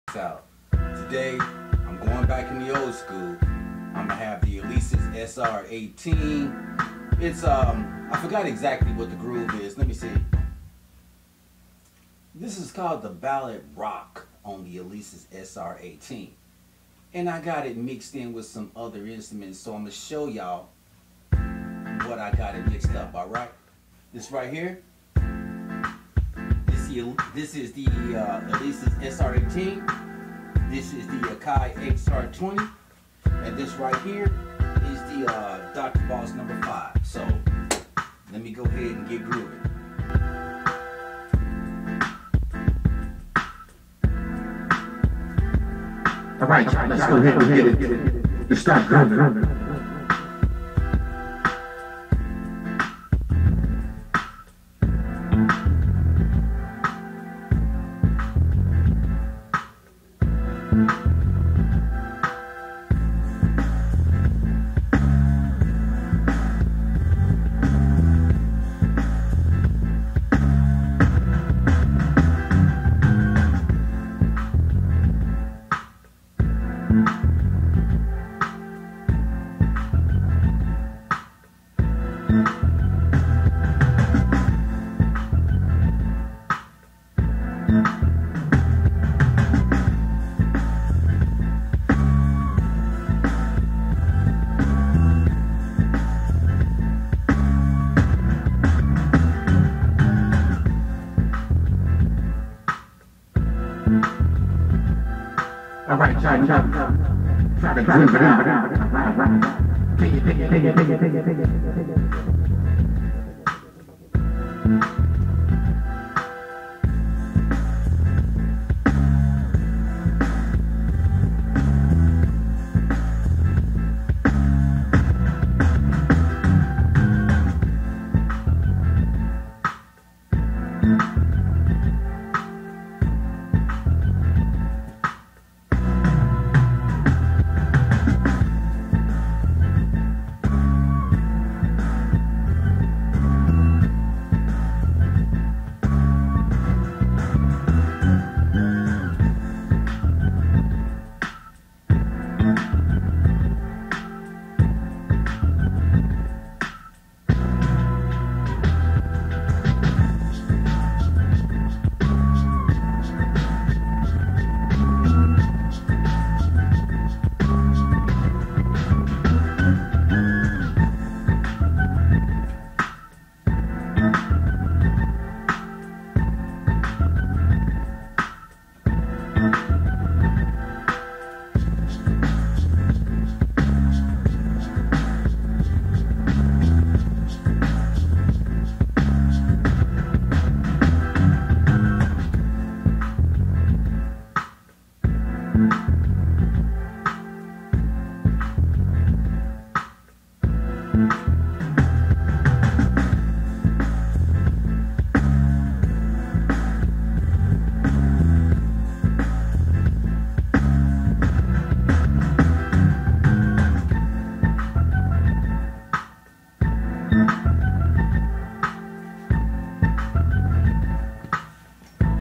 Jazzy funk groove
this was made with a drum machine in synchronization
The Alesis SR18 is sending midi time code out to around 5 instruments.